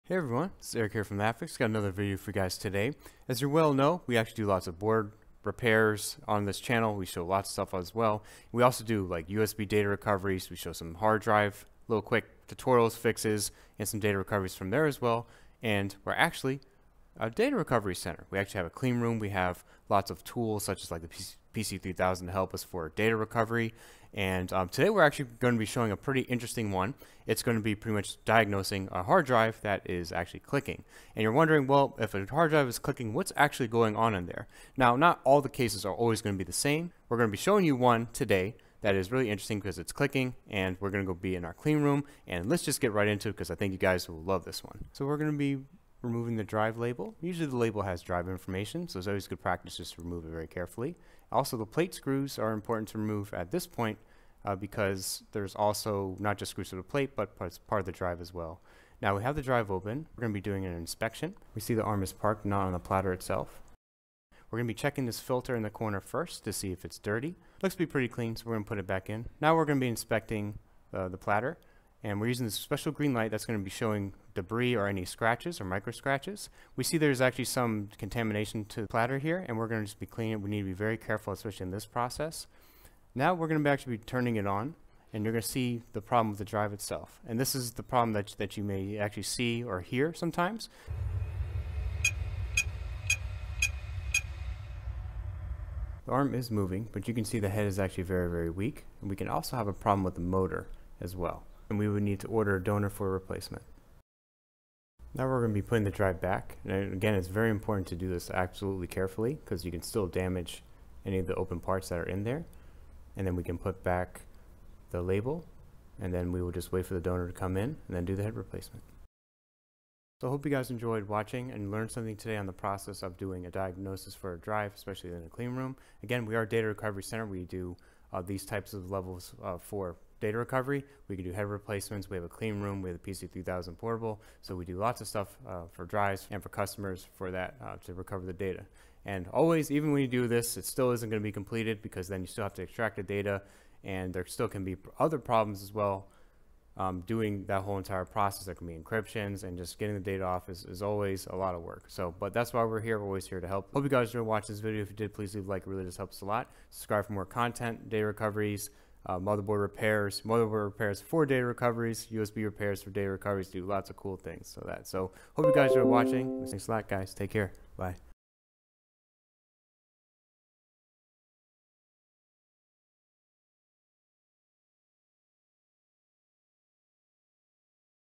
Opening and Diagnosing a Clicking WD Hard Drive
Hey everyone, In this video we will be Opening and Diagnosing a Clicking WD Hard Drive. We will be opening the hard drive in our clean room and guiding you  step-by-step on how to properly open and inspect the drive.